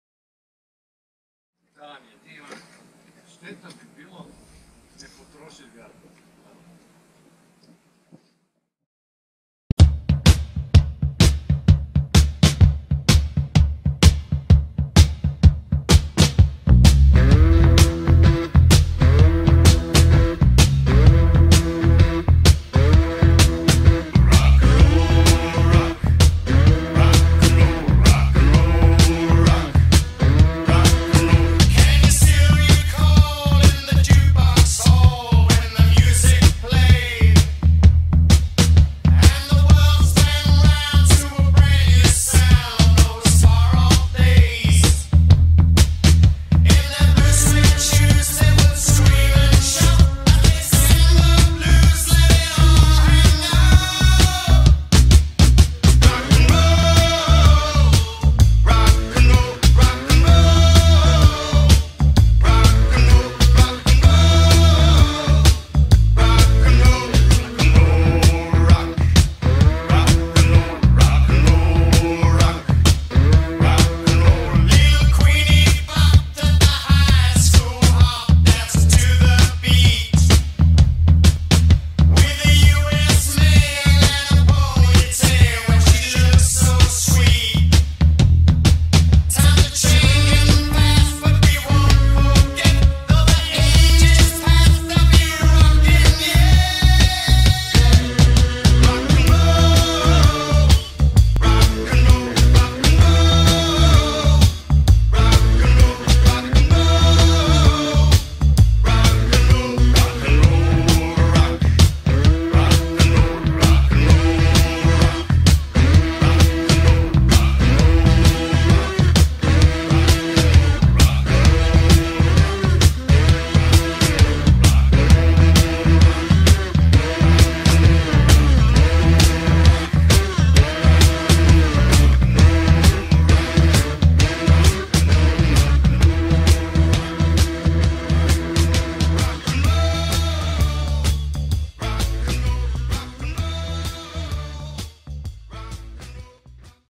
Rijeka/Kostrena Diving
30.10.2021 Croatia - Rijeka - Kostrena

Beautiful day for diving, a bit cold but still perfect
max temp 16 degrees